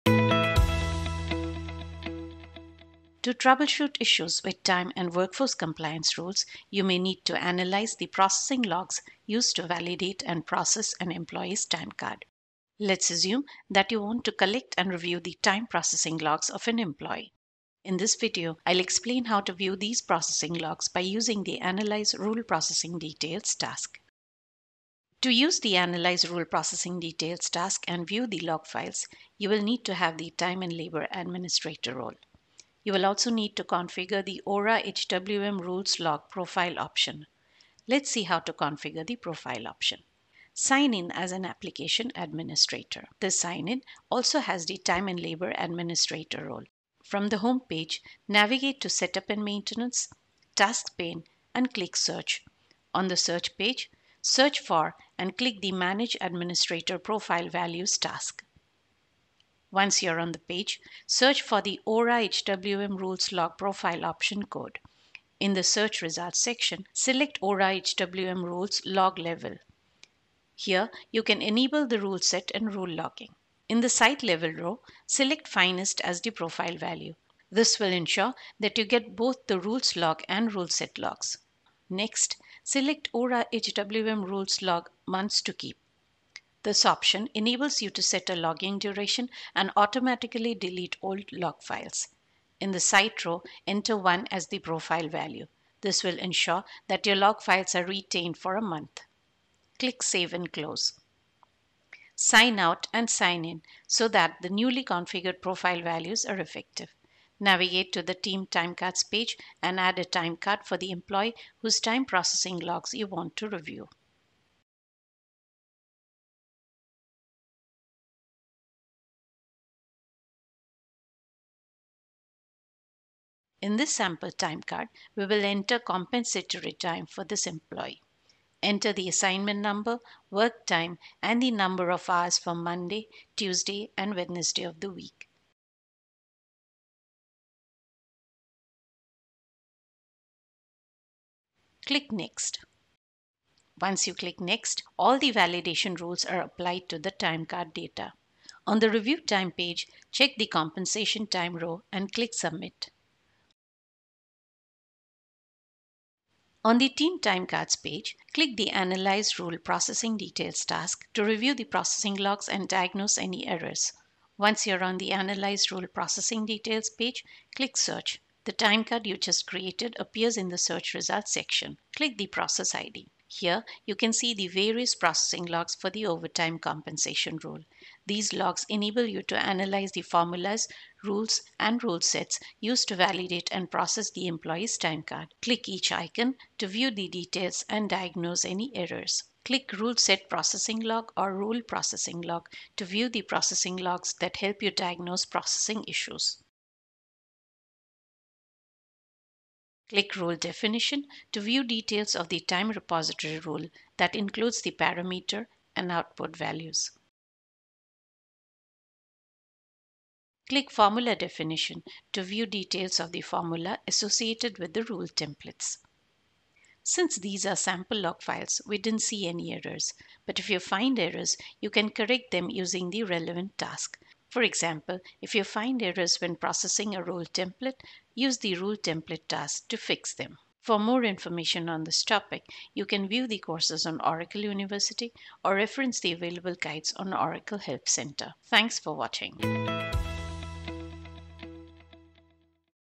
Time and Labor | How do I collect the processing logs for time rules?
Learn how to view the processing logs used to validate and process an employee’s time card.